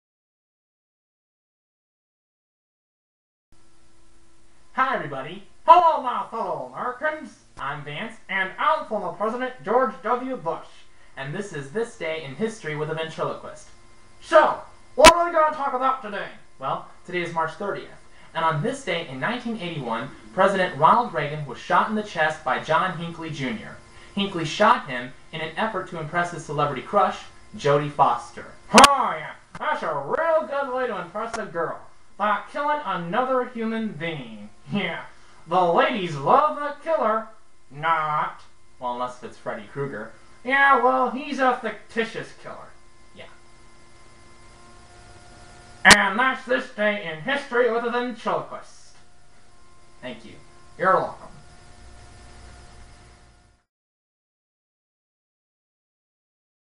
This Day In History With A Ventriloquist (March 30th)
Puppet Bush and I mention how on this day in 1981 Pres. Ronald Reagan was shot in the chest by John Hinckley, Jr. in an effort to impress his celebrity crush Jodi Foster.